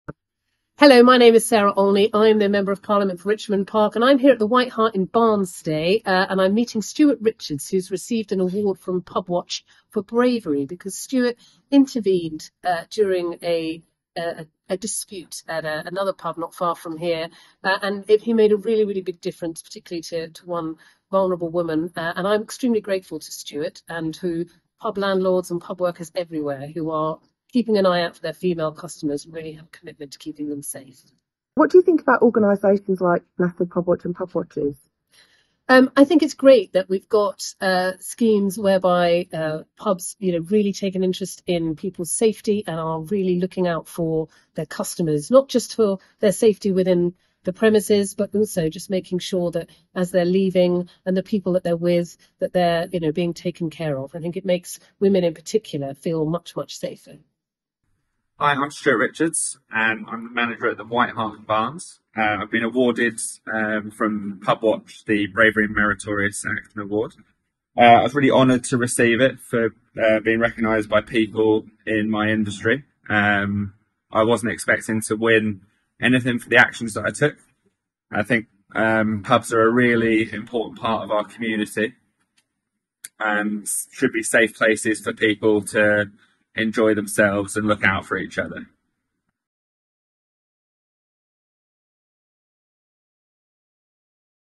Stuart Richards, Manager White Hart, Barnes
Stuart was awarded a National Pubwatch Bravery and Meritorious Conduct Award.